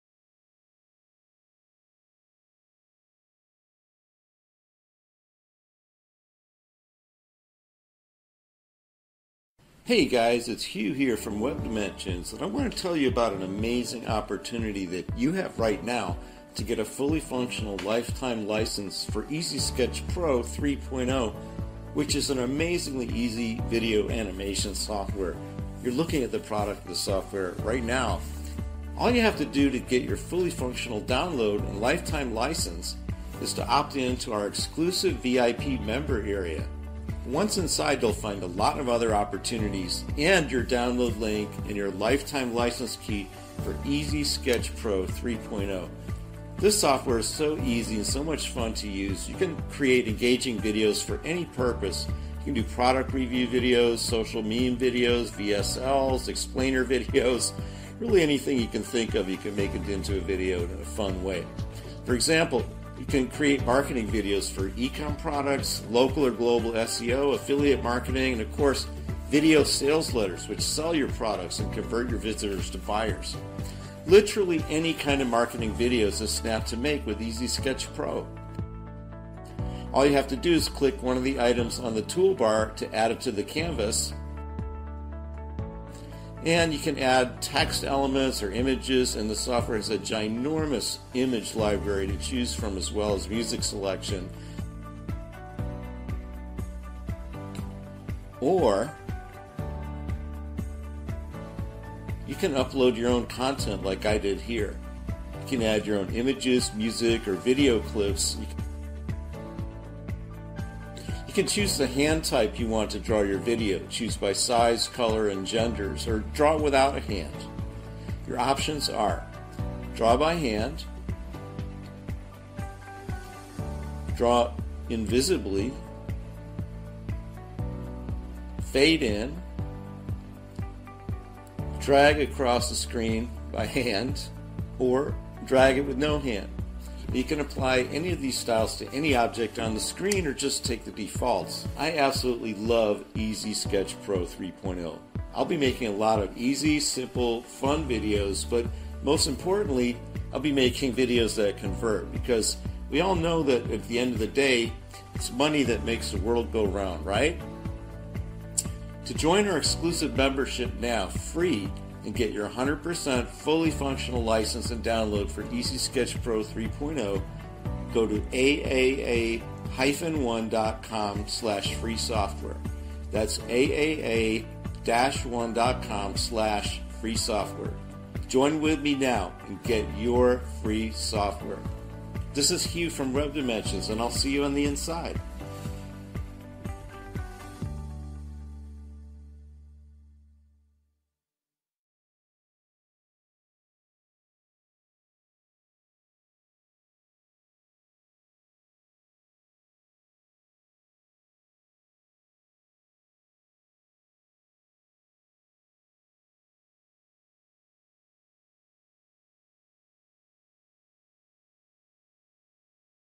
Video Maker Software Free Download
I wanted to take a few moments to tell you about this amazing video animation software tool that you can get for free right now if you hurry.

It's a 100% fully functional out of the box and all you have to do to get your permanent license key is opt in to create your own membership in the premium member area where you download the software.

Let's take a look at this cool software and how much fun it is to make quick, simple but professional looking videos so you can instantly be video marketing for any product, offer or website.

First off I want to bring to your attention that you can create videos in 2 different HD settings and one standard setting - it's your choice. I usually use 1280x720 for my videos (720p) but you can go up to 1920x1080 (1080p).

Once you start your project you have this drawing board here in the middle which is where you're going to position all your elements.

you can add images from the humongous library that comes with Easy Sketch Pro or you can add your own images from your computer and you can also add video to your project if you have clips you want to use.

Every time you add a new item to the canvas, it gets an order number. So what you want to do is order your elements in the order you want from 1 being the first one to appear, to whatever number of elements you have on the page.

you change the order, the size, the position and several other properties by double clicking on an element and then operating on it.

You can also add music by selecting from the provided selections or upload your own mp3s. You can also upload voice tracks, or even record your own voice to be a part of your video and accompany the visuals created by the software.

If you want every element on the canvas to be hand drawn, you can just drop things on the page one at a time, position them and you're ready to export your video.

If you want to spend a little more time creating more complex arrangements of timing, fade-ins and lingering before or after an object appears, you have all the controls to do that for each element.

Once you get finished with one scenario, or "slide", you can just add another slide for your next scene in the video and start all over. You can add an unlimited number of slides or scenes to your videos to literally get any message across in a very engaging way.

All in all I give Easy Sketch Pro ten stars out of ten for being easy to use, highly functional, and creating professional looking videos in a minimum amount of time and effort spent.

To get your FREE copy of Easy Sketch Pro, follow the links provided in this video.